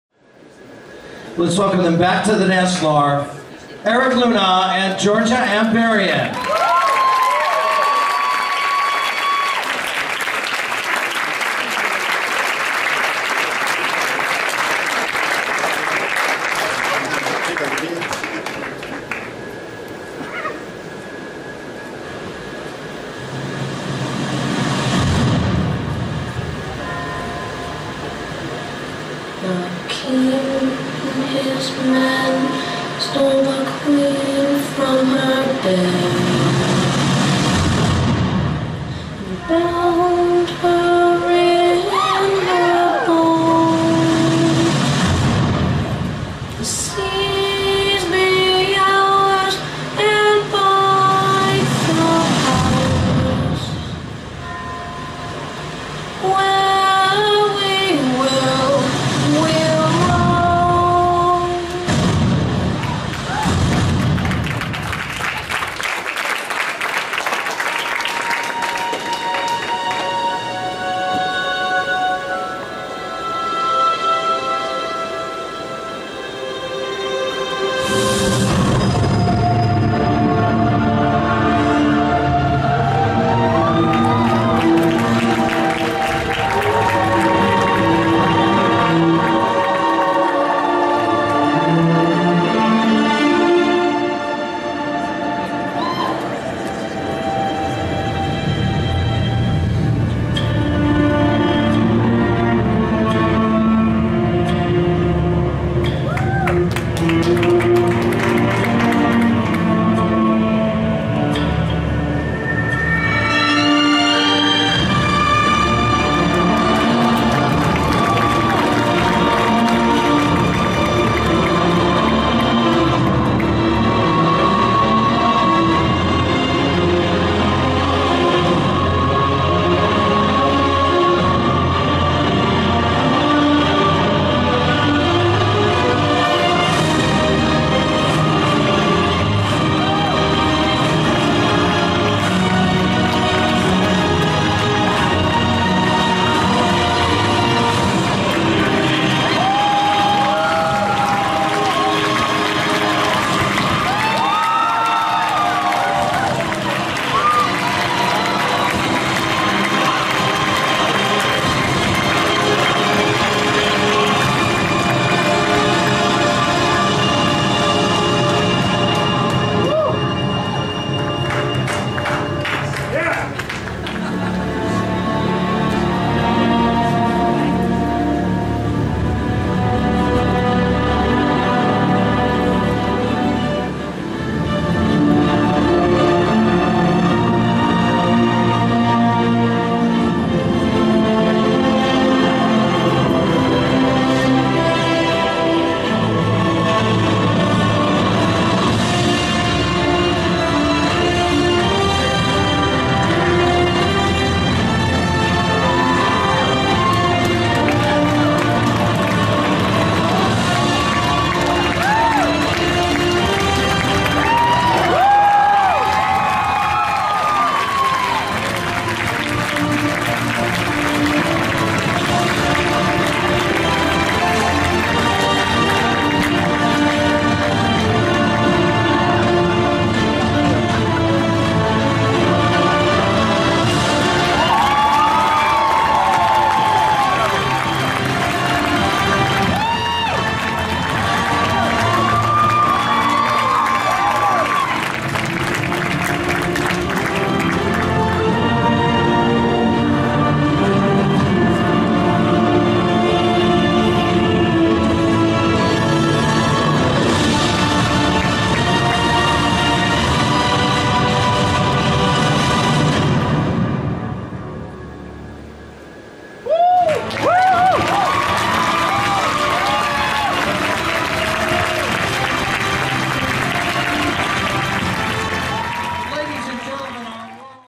Eric Luna & Georgia Ambarian at Trophy Ball 2011 Part 2
5 Time World Champions Dancing at Fred Astaire Wisconsin's Trophy Ball